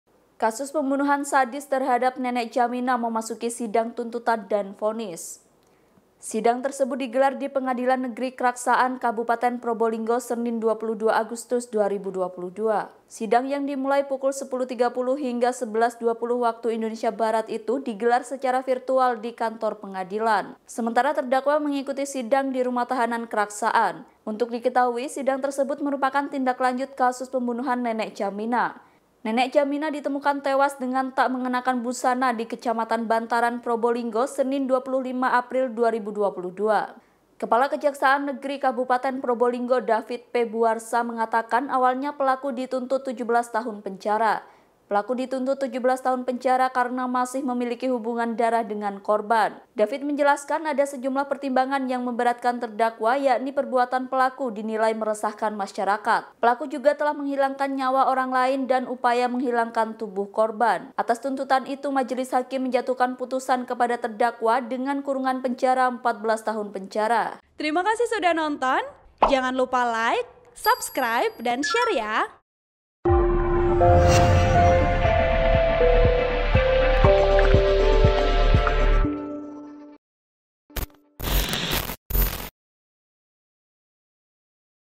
Pelaku Pembunuhan Sadis Nenek Jamina yang Ditemukan Tewas Tanpa Busana Divonis 14 Tahun Penjara
TRIBUN-VIDEO.COM - Kasus pembunuhan sadis seorang nenek memasuki sidang tuntutan dan vonis.

Sidang tersebut digelar di Pengadilan Negeri Kraksaan, Kabupaten Probolinggo, Senin (22/8/2022).

Sidang yang dimulai pukul 10.30 WIB hingga 11.20 WIB itu digelar secara virtual di kantor pengadilan.

Sementara terdakwa mengikuti sidang di Rumah Tahanan Kraksaan.

Untuk diketahui sidang tersebut merupakan tindak lanjut kasus pembunuhan nenek Jamina.

Nenek Jamina ditemukan tewas dengan tak mengenakan busana di Kecamatan Bantaran, Probolinggo, Senin (25/4/2022).

Kepala Kejaksaan Negeri (Kajari) Kabupaten Probolinggo David P Buarsa mengatakan pelaku terancam 17 tahun penjar.

Pelaku dituntut 17 tahun penjara karena masih memiliki hubungan darah dengan korban.

David menjelaskan, ada sejumlah pertimbangan yang memberatkan terdakwa, yakni perbuatan pelaku dinilai meresahkan masyarakat.

Pelaku juga telah menghilangkan nyawa orang lain dan ada upaya menghilangkan tubuh korban.

Atas tuntutan itu, majelis hakim menjatuhkan putusan kepada terdakwa dengan kurungan penjara 14 tahun penjara. (Tribun-Video.com/Kompas.com)

Artikel ini telah tayang di Kompas.com dengan judul "Kasus Pembunuhan Sadis Nenek di Probolinggo, Pelaku Divonis 14 Tahun Penjara", Klik untuk baca
Penulis : Kontributor Probolinggo, Ahmad Faisol
Editor : Dheri Agriesta

Host: Umi Wakhidah
VP: Indra Imawan